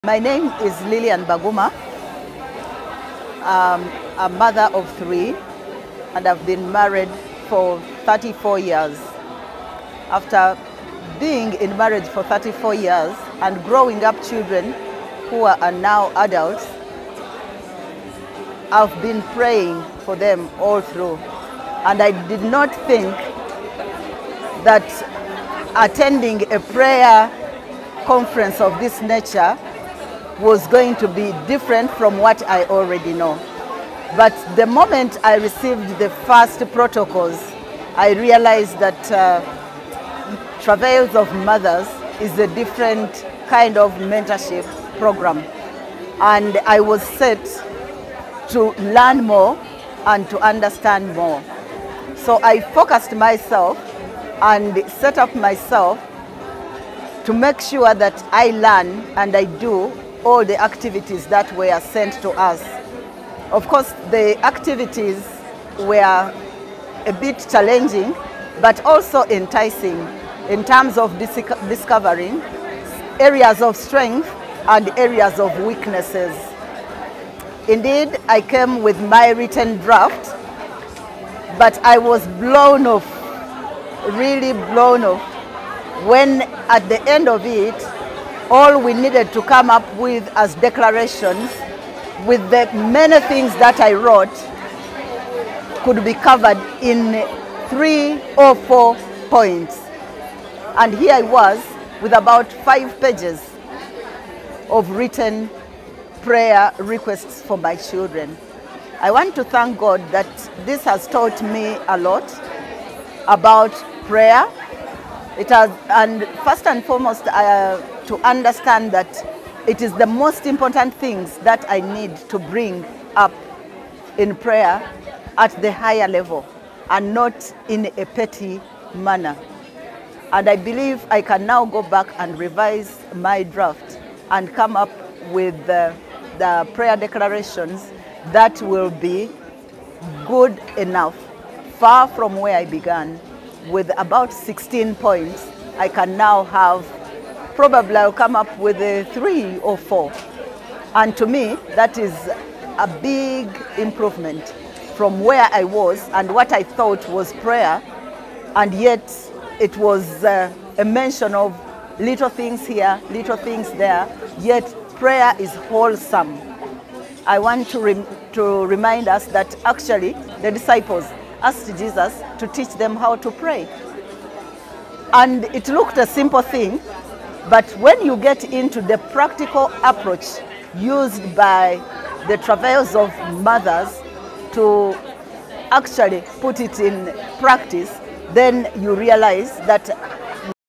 High end versus petty finished product prayers was Auntie Lillian’s biggest lesson!
Prayer is not a shopping spree! There are dimensions and hierarchies. A simple look at the Lord’s Prayer will easily reveal that. Have you learnt the most cases you receive nothing from God because you pray finished product prayers only? Auntie Lilian, married 35 whole years took away some lessons from Saturday. We pray you too do as you come to Nairobi for our Nairobi edition.